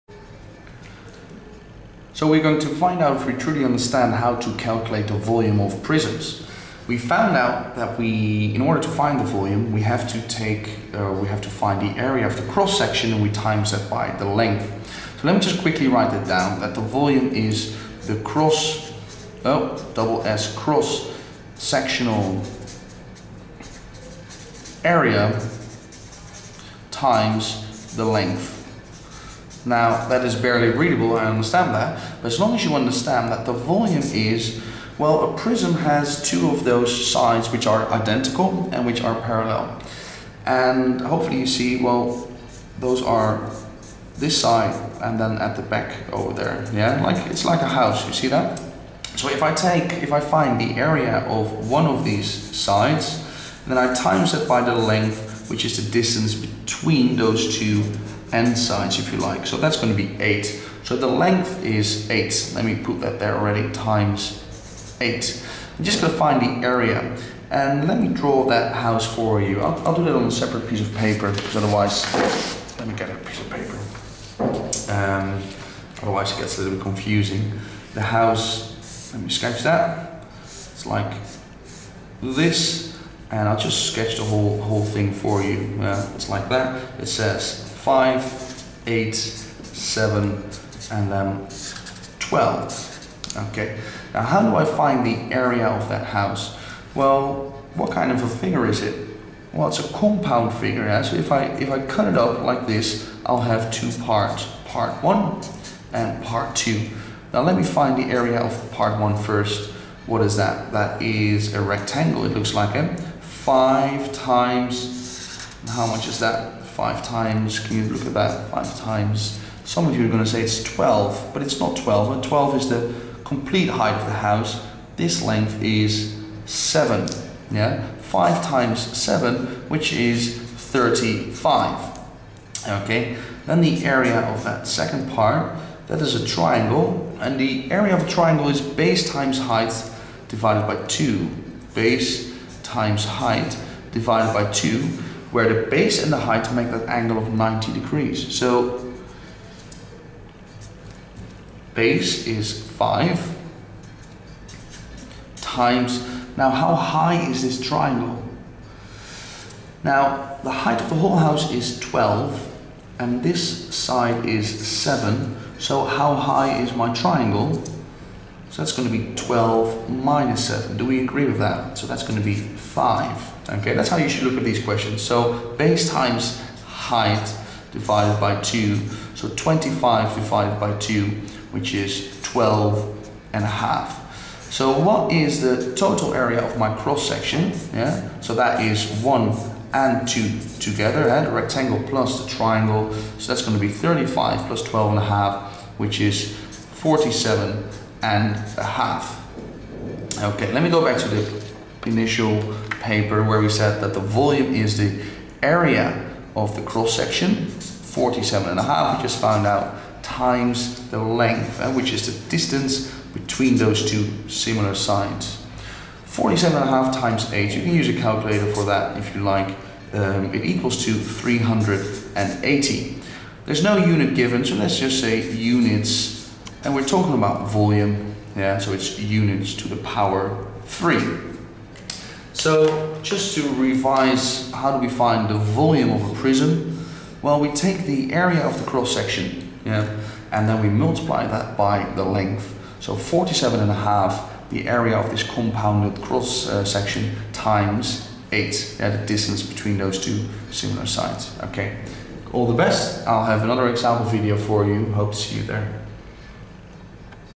How to calculate Volume of prism 2 - Mensuration Help - ExplainingMaths.com IGCSE and GCSE maths
We will calculate the volume of a prism.  Step by step I will show you what you need to take into account. This maths video will explain this IGCSE and GCSE mathematical topic to you by providing example questions.  You can use these maths activities for your math revision to do better on your IGCSE GCSE exam paper. Check for more maths help and FREE resource, activities and worksheets.